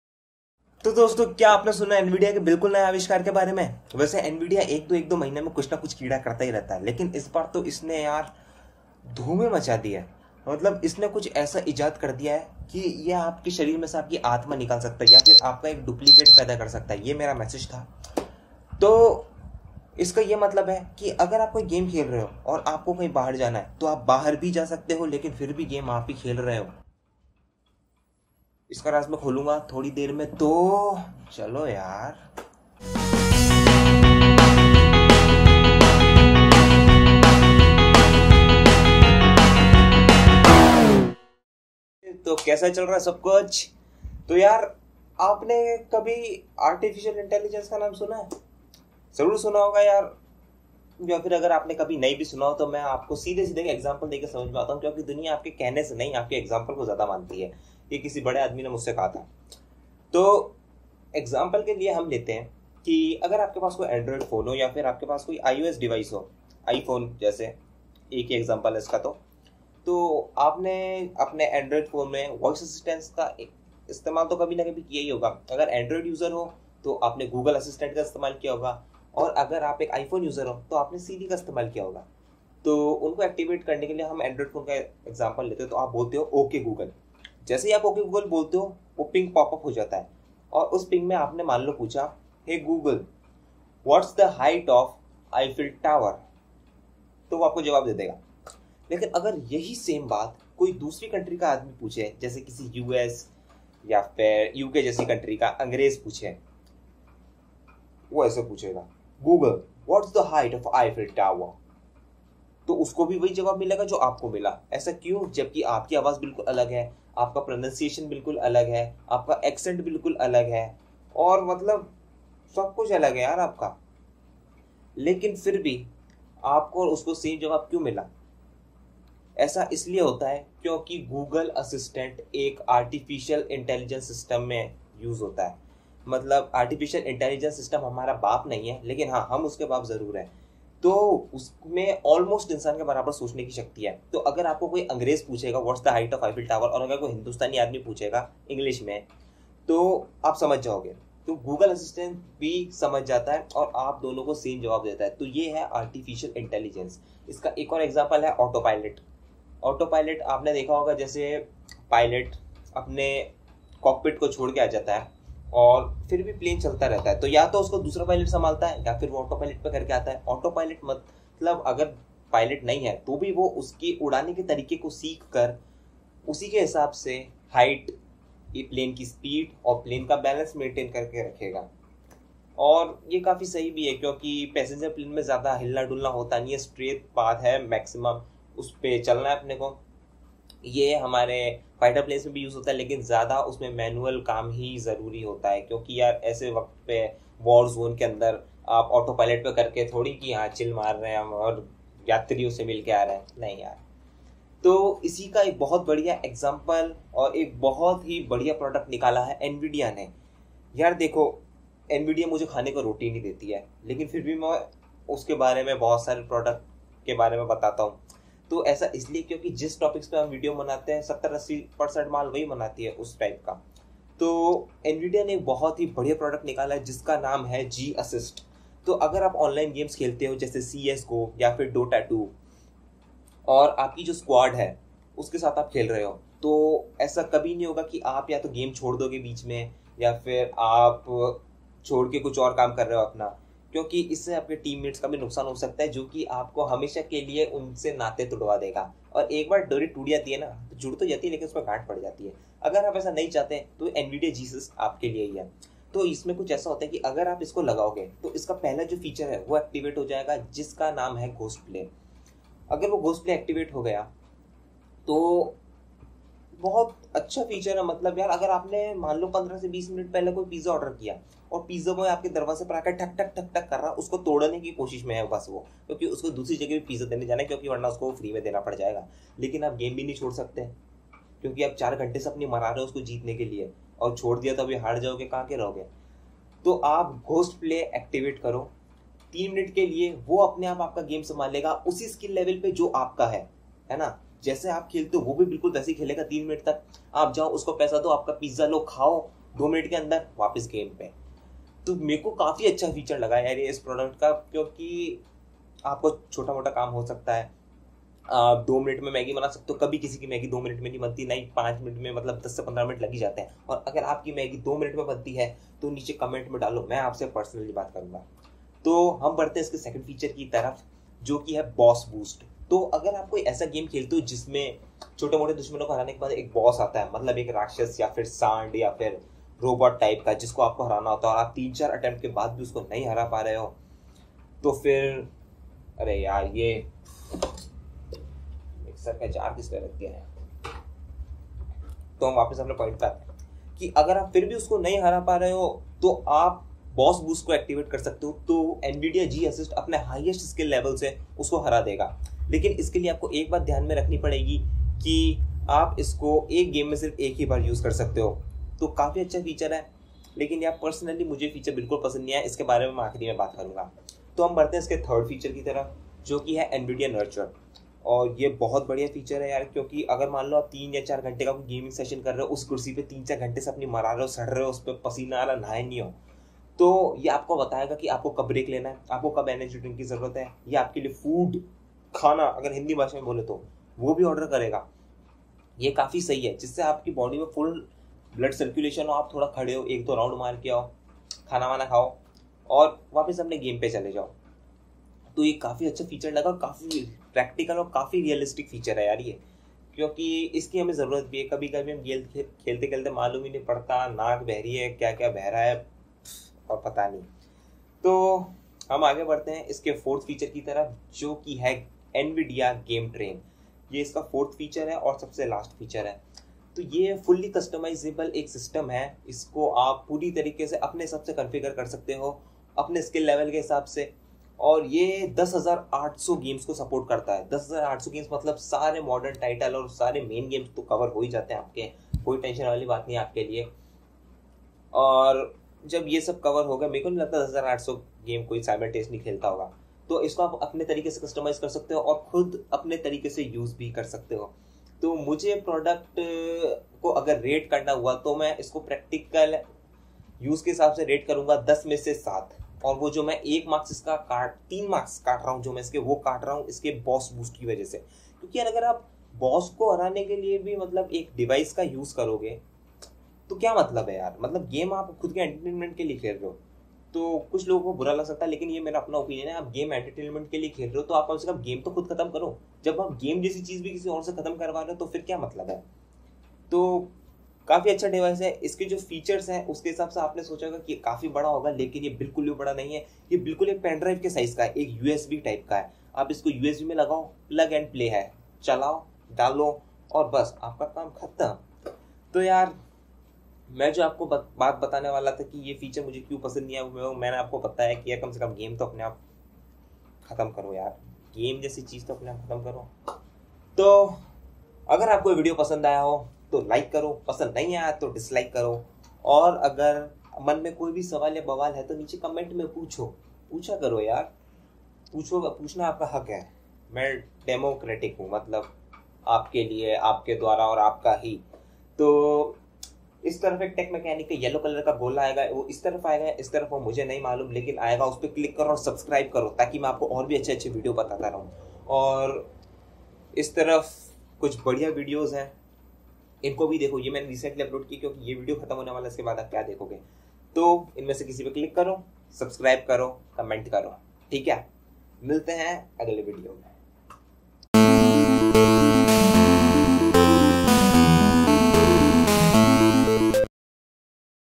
[HINDI] Nvidia GeForce GTX G-Assist Overview || Now play game without doing anything
This is the first overview of Nvidia GeForce GTX G-Assist in Hindi. Do ask me any doubt in comments section.

STAY UPDATED AND STAY AWESOME, Which you already are...